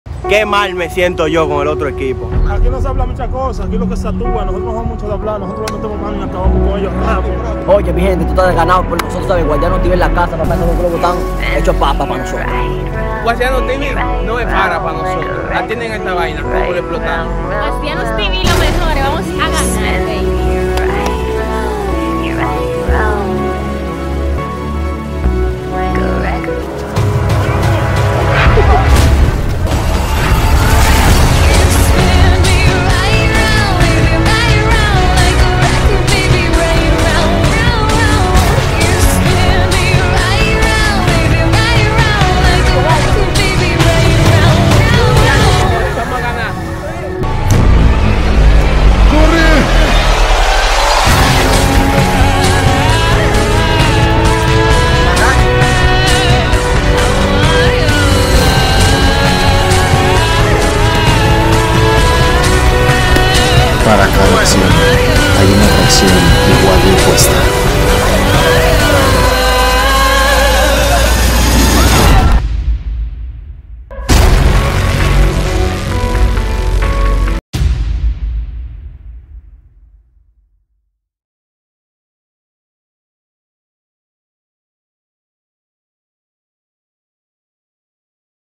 TIENES QUE VER ESTO | @AmarilloHD1 vs @ShowdeCalle | La Batalla Trailer
La Batalla entre Uasdianos Tv y Show de Calle es inminente...

Te recomendamos que estes suscrito a ambos canales...

@AmarilloHD1 vs @ShowdeCalle